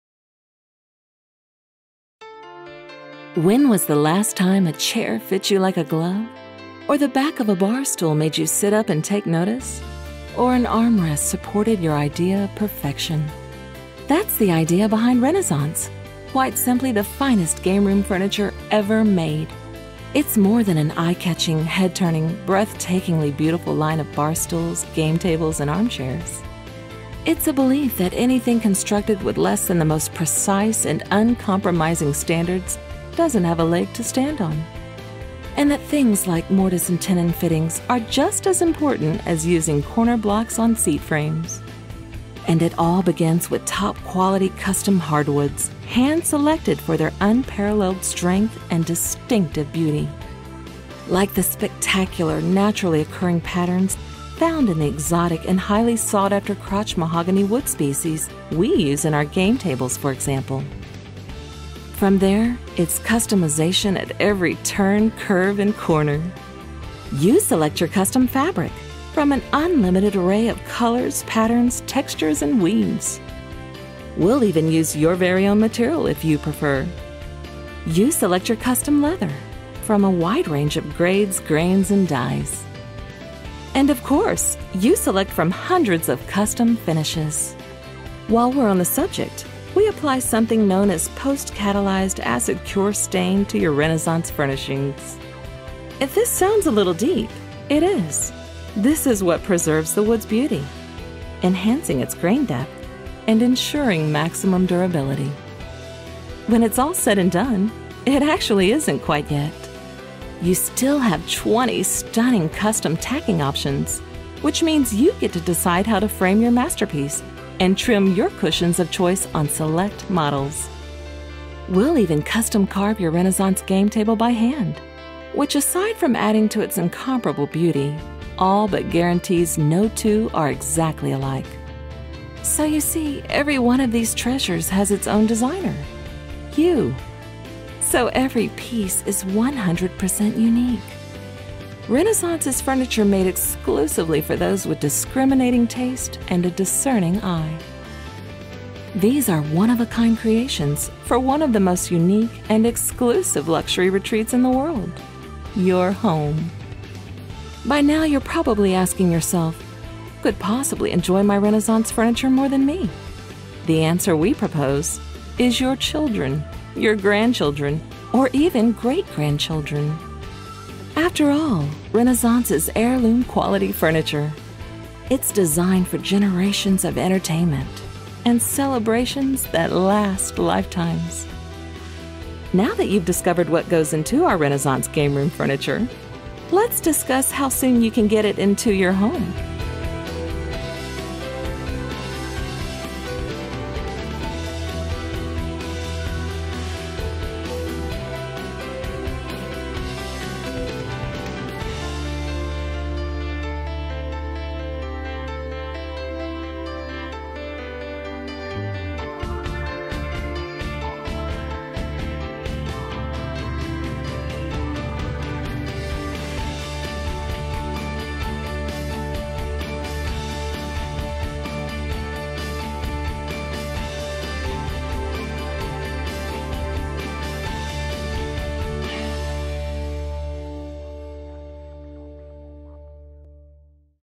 Renaissance Fine Furniture
Quite simply, Renaissance is the finest game room furniture ever made. It's more than an eye-catching, head-turning, breathtakingly beautiful line of barstools, game tables and armchairs. It's a belief that anything constructed with less than the most precise and uncompromising standards doesn't have a leg to stand on.

You select your custom fabric -- from an unlimited array of colors, patterns, textures and weaves. We'll even use your very own material, if you prefer.
You select your custom leather -- from a wide range of grades, grains and dyes. And, of course, you select from hundreds of custom finishes.

Renaissance is furniture made exclusively for those with discriminating taste and a discerning eye. These are one-of-a-kind creations for one of the most unique and exclusive luxury retreats in the world: your home.